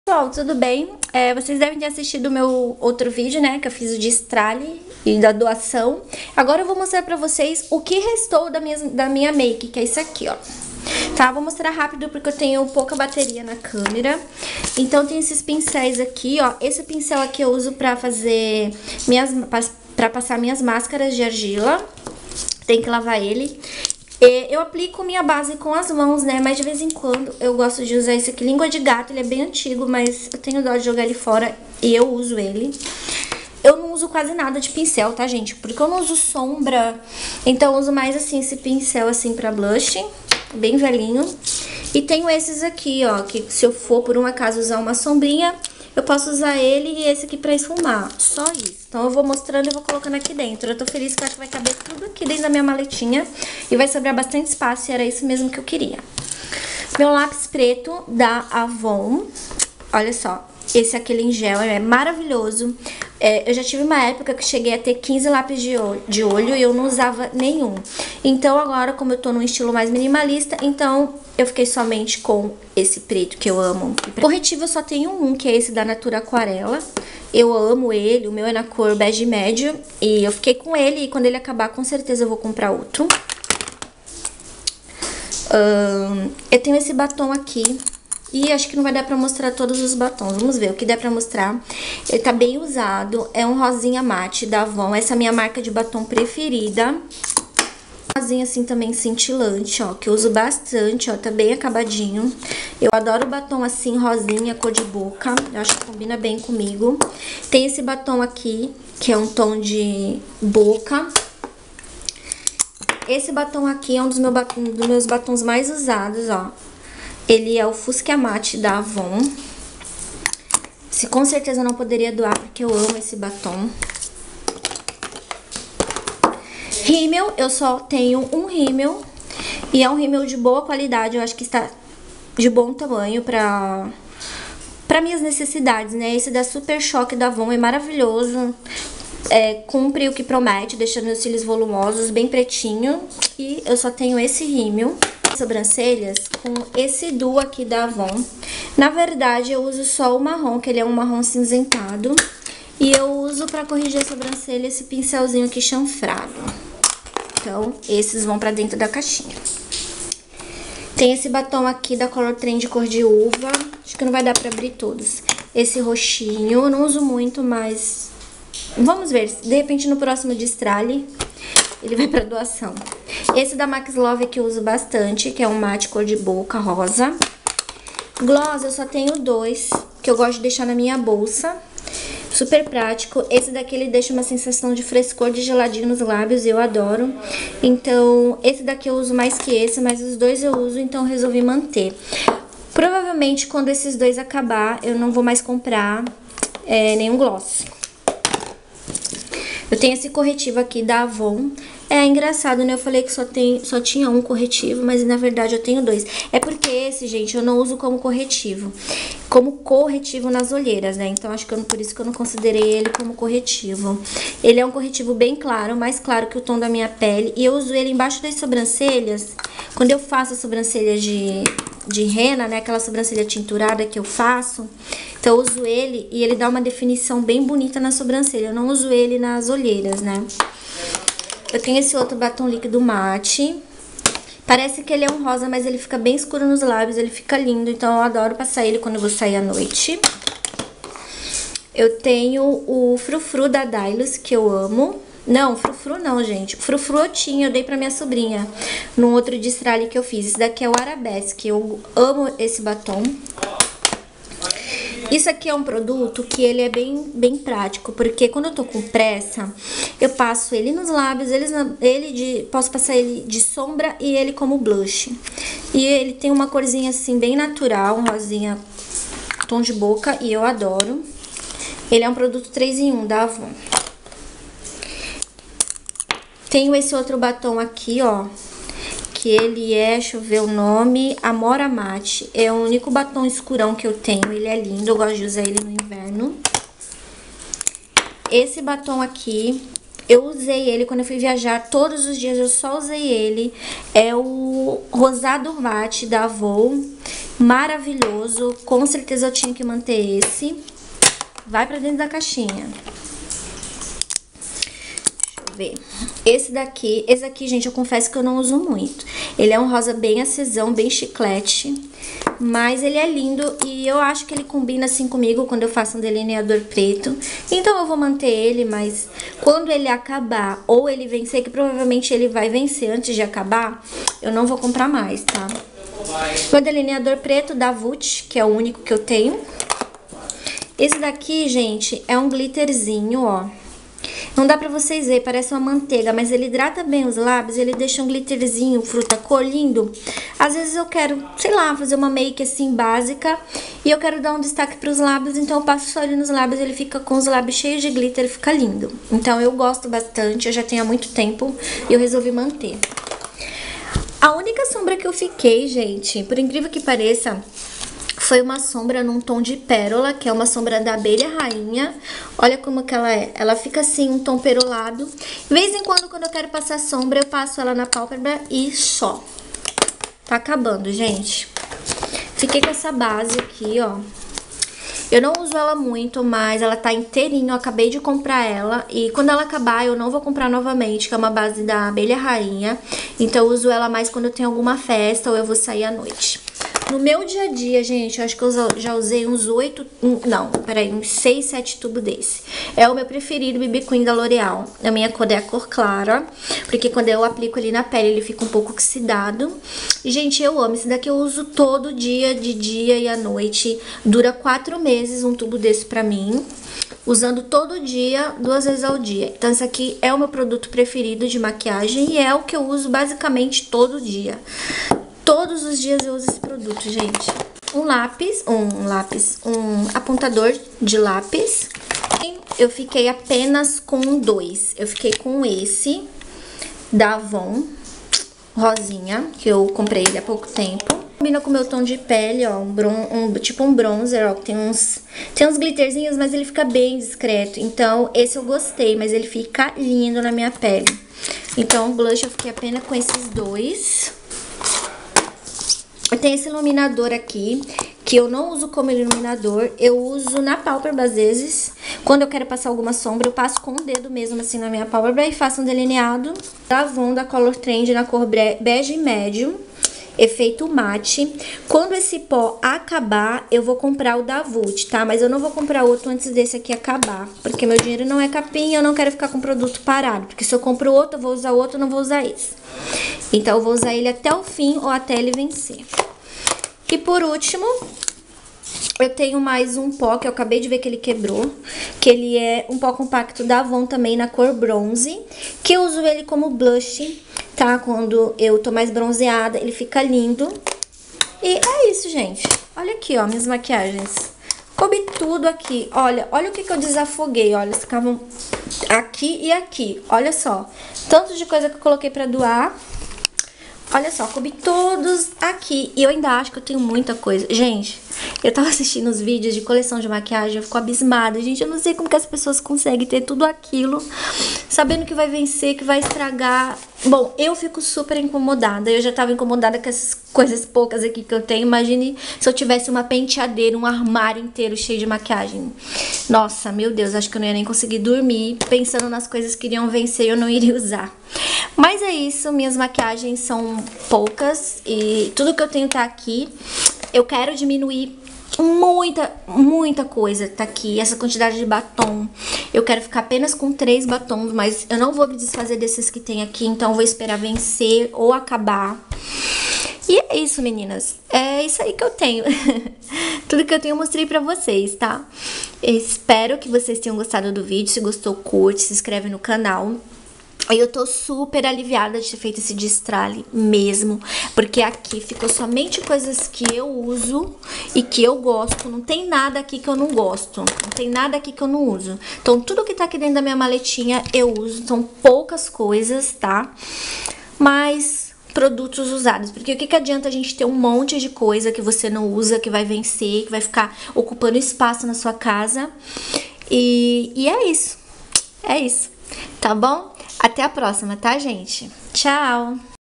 Minimalismo -- Todas as Minhas Maquiagens -- MENOS É MAIS !!!
Nesse vídeo eu mostro todas as minhas maquiagens , estou reduzindo ao máximo e ficando somente com o necessário para o meu consumo , beijo a todos .